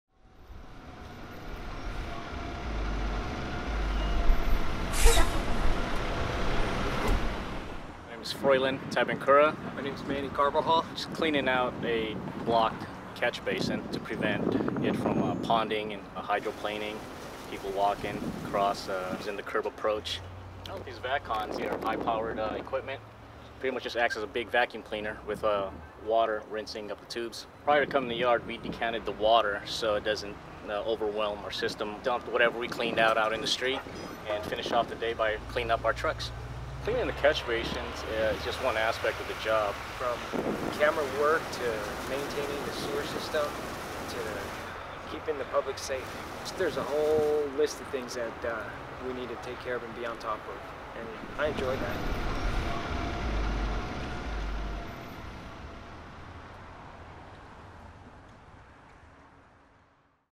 Catch Basin Cleaning
Our Vac-Con trucks help keep catch basins and storm drains clear of trash and debris throughout San Francisco. Our crews are standing by to explain how this truck helps keep our sewer system flowing properly.